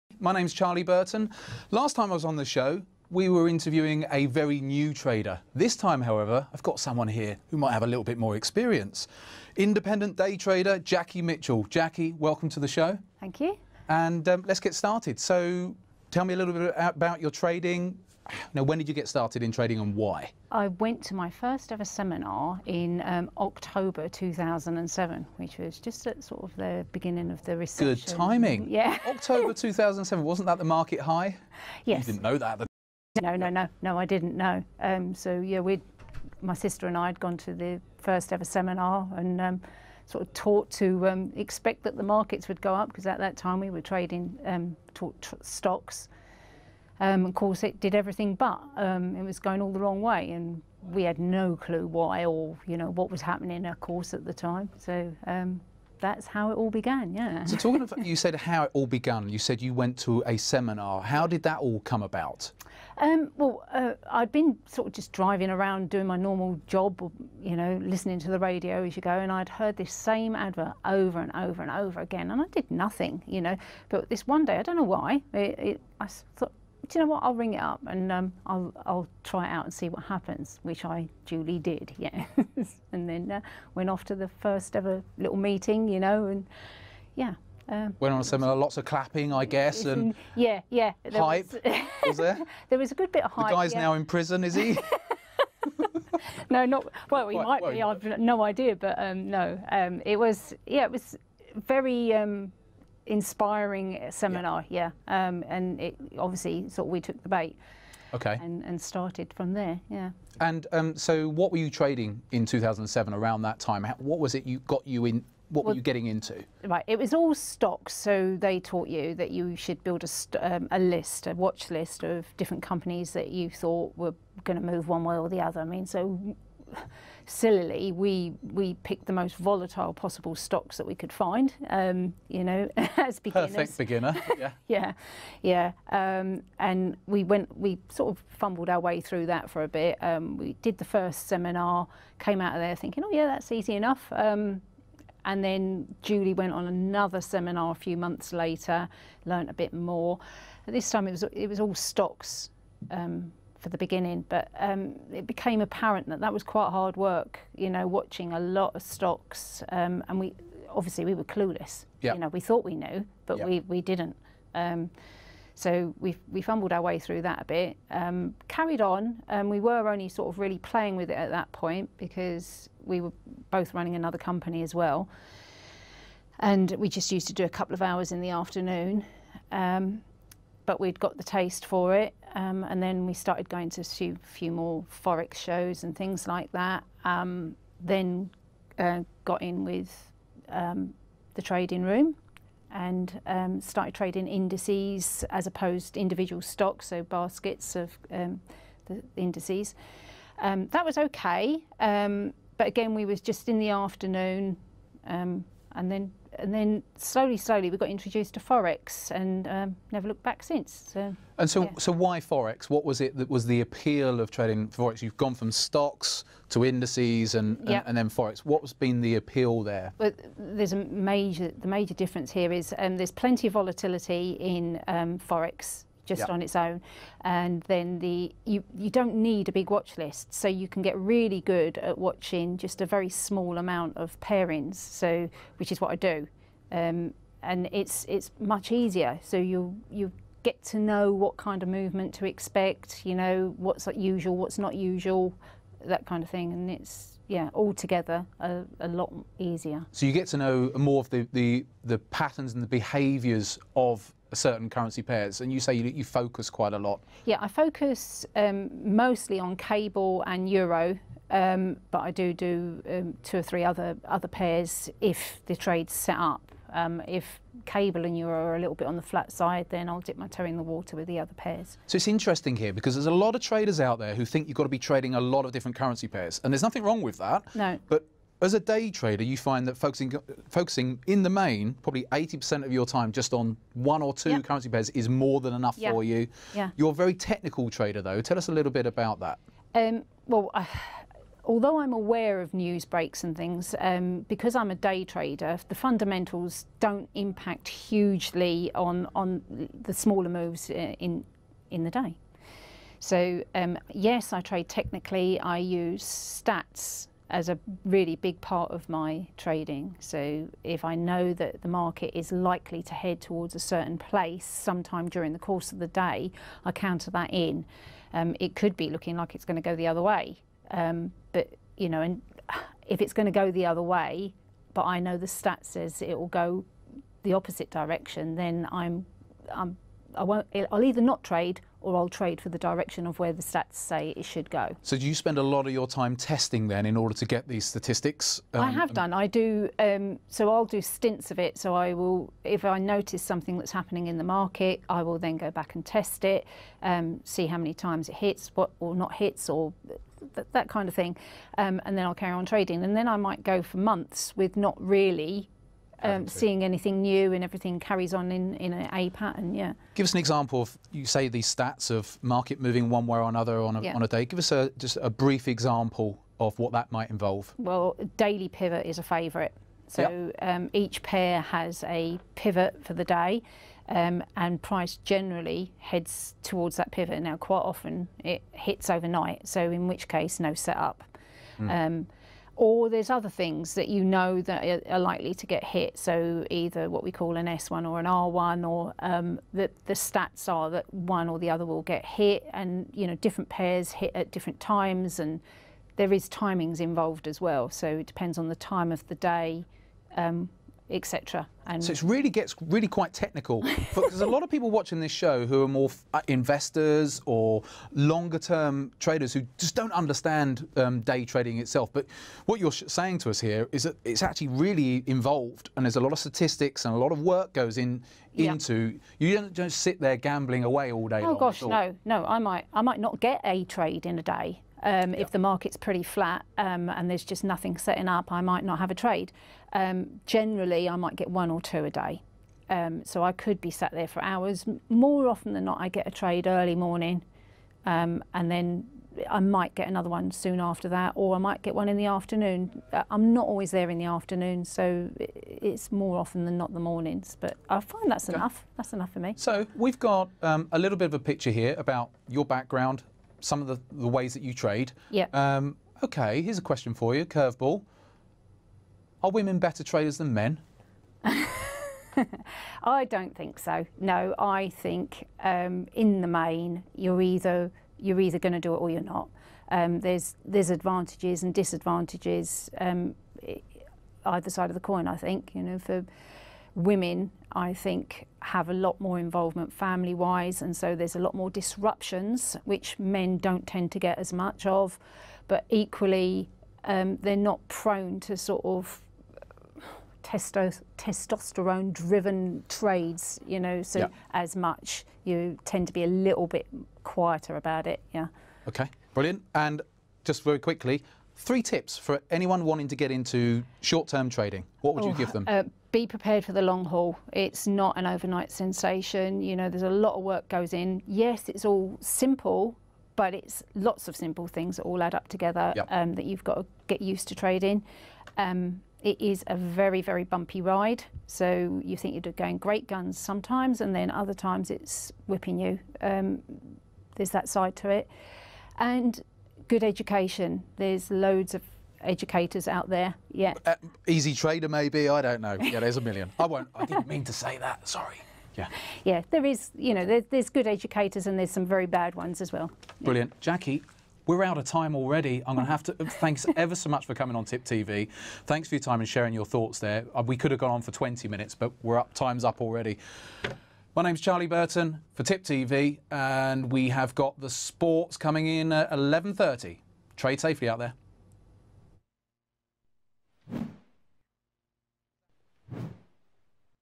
Interviewing Jackie Mitchell: Day Trading Currencies - Strategies and Tips
Charlie Burton interviews Jackie Mitchell to discover how she day trades plus her advice to anyone starting out.  I attended a seminar in October 2007 - that was the market high but of course Jackie had no clue that the markets were about to reverse.  At the time it was all stocks so we would pick up the most volatile stocks we could find then... After stocks I went to indices and finally forex - what's been the appeal here.  There is plenty of volatility in forex and its much easier, you just have to focus on a handful of forex pairs.  I mostly focus on Cable and Euro although I do 2 or 3 other pairs if the trade setups allow.  What are your tips for anyone wanting to get into short term trading?